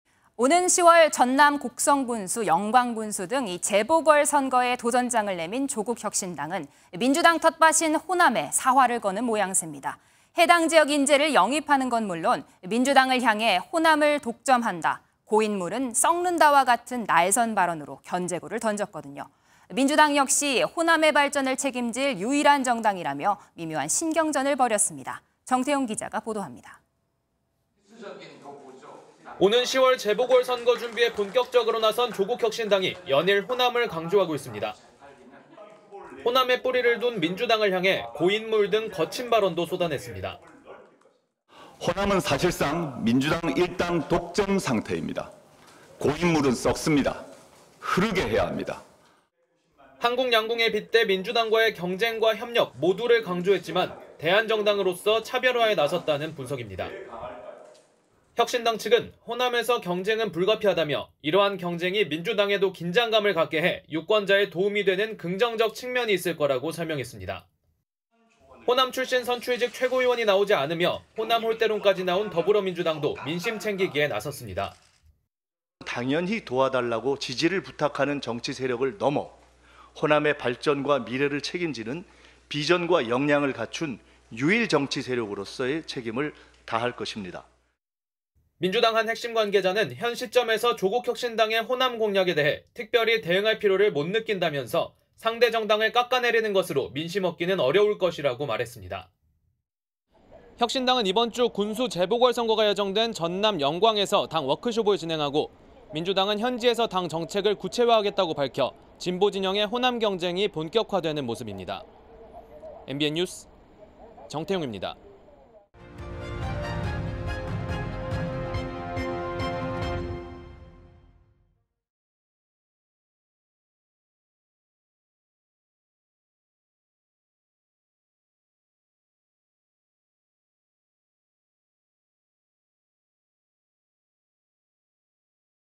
조국, 호남 본격 승부수 "민주당 호남 독점, 고인물 흐르게 해야" [MBN 뉴스7]
【 앵커멘트 】
  오는 10월 전남 곡성군수·영광군수 등 재보궐 선거를 앞두고, 첫 지방자치단체장에 도전하는 조국혁신당은 민주당 텃밭인 호남에 사활을 거는 모습입니다.
  해당 지역 인재를 영입하는 건 물론 민주당에 대해 "호남을 독점한다", "고인 물은 썩는다"와 같은 거센 발언으로 견제구를 던졌거든요.
  민주당 역시 호남의 발전을 책임질 유일한 정당이라며 미묘한 신경전을 벌였습니다.
  정태웅 기자가 보도합니다.

【 기자 】
  오는 10월 재보궐선거 준비에 본격적으로 나선 조국혁신당이 연일 호남을 강조하고 있습니다.

  호남에 뿌리를 둔 민주당을 향해 '고인물' 등 거친 발언도 쏟아냈습니다.

▶ 인터뷰 : 조 국 / 조국혁신당 대표
- "호남은 사실상 민주당 일당 독점 상태입니다. 고인 물은 썩습니다. 흐르게 해야 합니다."

  한국 양궁에 빗대 민주당과의 경쟁과 협력 모두를 강조했지만, 대안 정당으로서 차별화에 나섰다는 분석입니다.

  혁신당 측은 호남에서 경쟁은 불가피하다며 이러한 경쟁이 민주당에도 긴장감을 갖게 해 유권자에 도움이 되는 긍정적 측면이 있을 거라고 설명했습니다.

  호남 출신 선출직 최고위원이 나오지 않으며 '호남 홀대론'까지 나온 더불어민주당도 민심 챙기기에 나섰습니다.

▶ 인터뷰 : 김민석 / 더불어민주당 최고위원
- "(호남에) 당연히 도와달라고 지지를 부탁하는 정치세력을 넘어 호남의 발전과 미래를 책임지는 비전과 역량을 갖춘 유일 정치 세력으로서의 책임을 다할 것입니다."

  민주당 한 핵심관계자는 현 시점에서 조국혁신당의 호남 공략에 대해 특별히 대응할 필요를 못 느낀다면서, '상대 정당을 깎아내리는 것으로 민심 얻기는 어려울 것'이라고 말했습니다.

▶ 스탠딩 : 정태웅 / 기자
- "혁신당은 이번주 군수 재보궐선거가 예정된 전남 영광에서 당 워크숍을 진행하고, 민주당은 현지에서 당 정책을 구체화하겠다고 밝혀 진보진영의 호남경쟁이 본격화되는 모습입니다. MBN뉴스 정태웅입니다."

  영상취재 : 임채웅 기자
  영상편집 : 송현주
  그래픽 : 양문혁